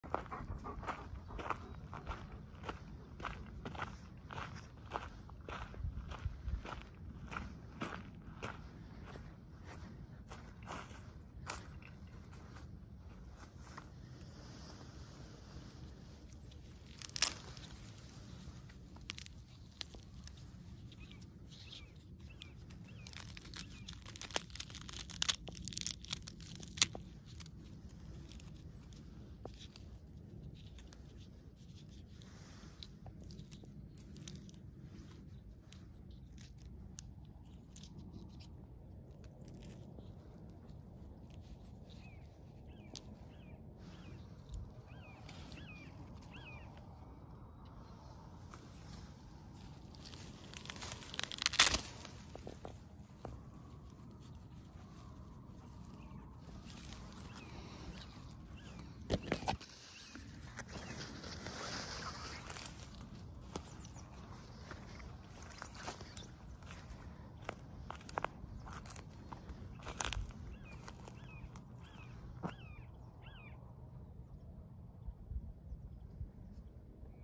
Pomegranates are ready
These will get picked this week for juice. The color is gorgeous!